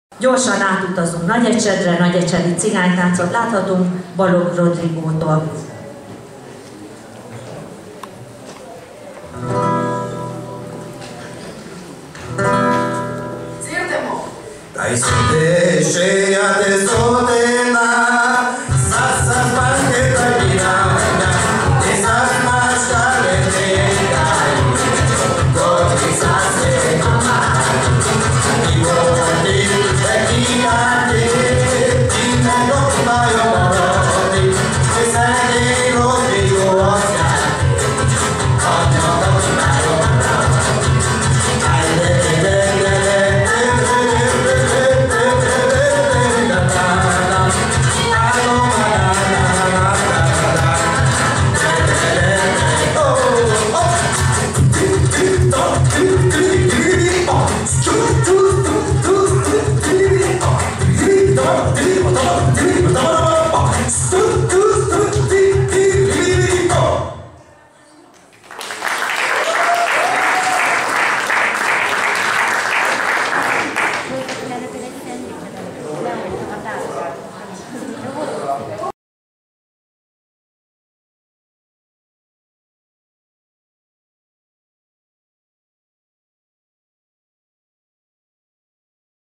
Balogh Rodrigó, Nyíregyháza, 2019. március 9.
Balogh Rodrigó, Szólótáncverseny, Nyíregyháza, 2019. március 9.
„A KÁRPÁT- MEDENCÉBEN ÉLEK” Szólótáncverseny
HELYSZÍN: NYÍREGYHÁZI EVANGÉLIKUS KOSSUTH LAJOS GIMNÁZIUM
Zene: Rostás Család